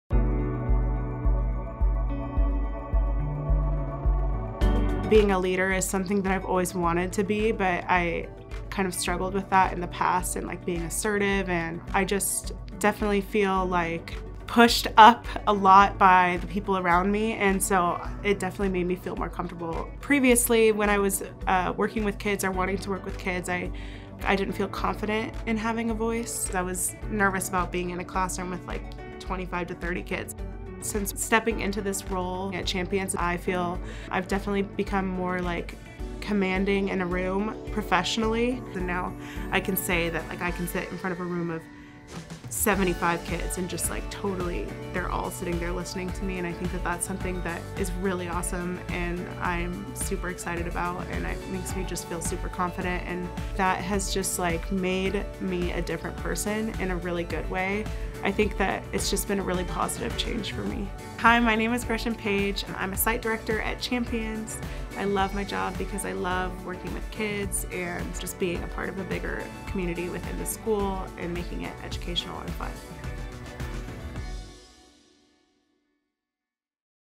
Meet Gretchen - A Champions Site Director
Gretchen acts as the bridge between her school and the Champions program. She makes sure things run smoothly on a daily basis, and provides kids, teachers, and parents with additional support as needed.